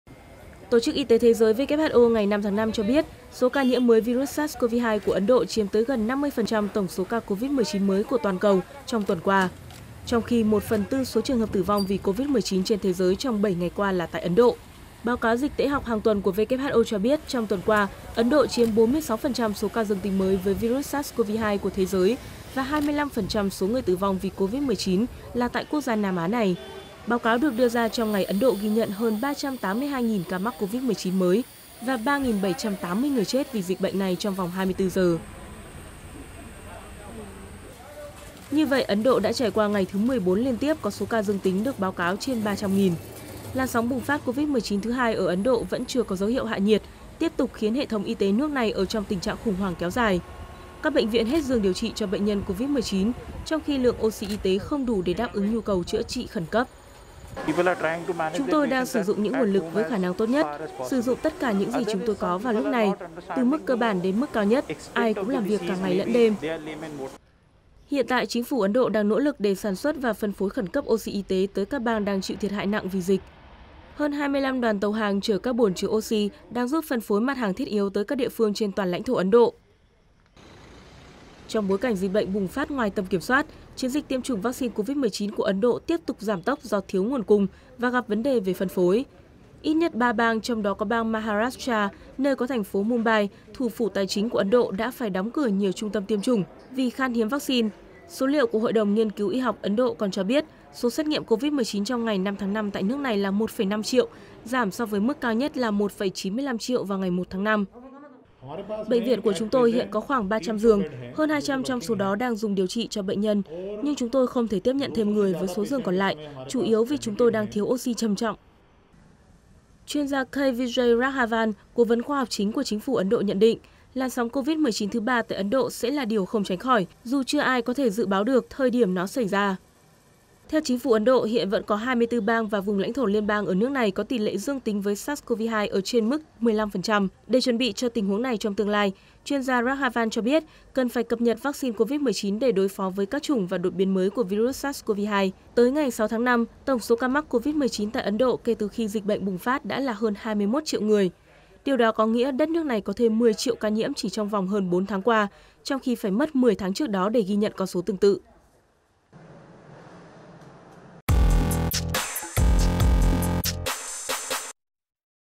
Ấn Độ chiếm gần một nửa số ca Covid-19 mới của thế giới tuần qua
Ấn Độ chiếm gần một nửa số ca Covid-19 mới của thế giới tuần qua, nguy cơ đối diện làn sóng dịch thứ ba
Tổ chức Y tế Thế giới (WHO) ngày 5/5 cho biết, số ca nhiễm mới virus SARS-CoV-2 của Ấn Độ chiếm tới gần 50% tổng số ca Covid-19 mới của toàn cầu trong tuần qua. Trong khi, 1/4 số trường hợp tử vong vì Covid-19 trên thế giới trong 7 ngày qua là tại Ấn Độ.